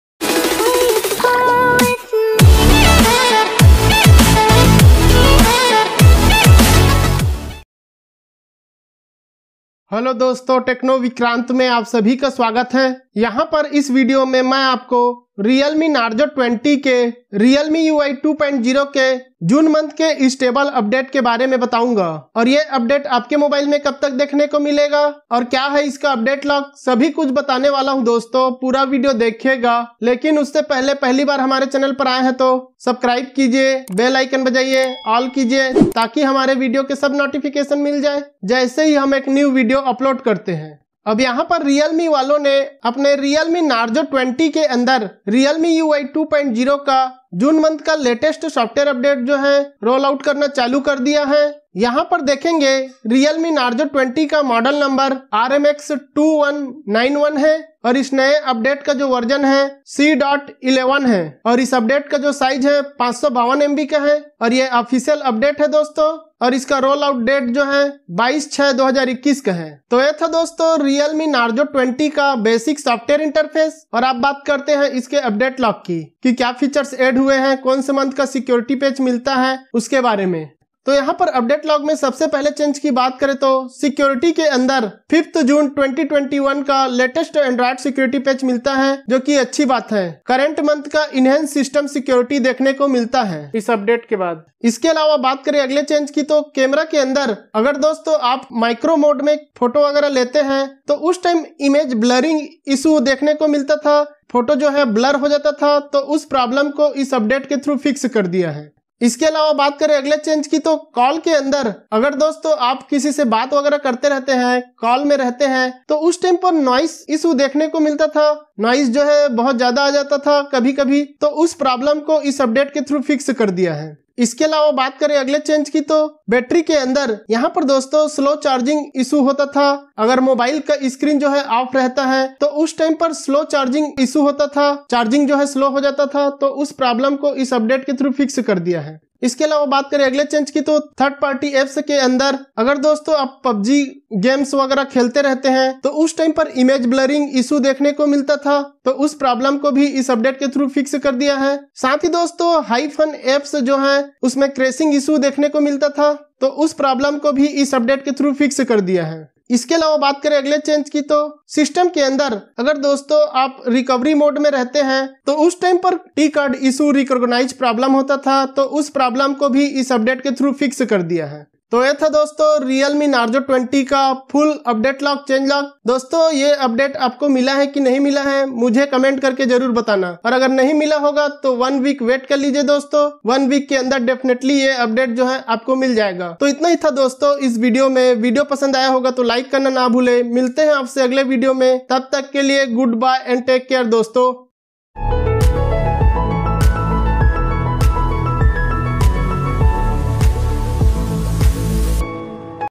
Realme Narzo 20 Realme UI 2.0 C.11 Stable Update | Narzo 20 June C.11 Update Quick Review
Dosto is video me maine aap logo ko Realme Narzo 20 ke Realme UI 2.0 ke June month ke New Software Update C.11 ke bare me bataya hu... June Update C.11 me Realme UI 2.0 or Android 11 ke New Features add hua or kuch known bugs fixed kiye hai...Umeed hai aapko video pasand aayega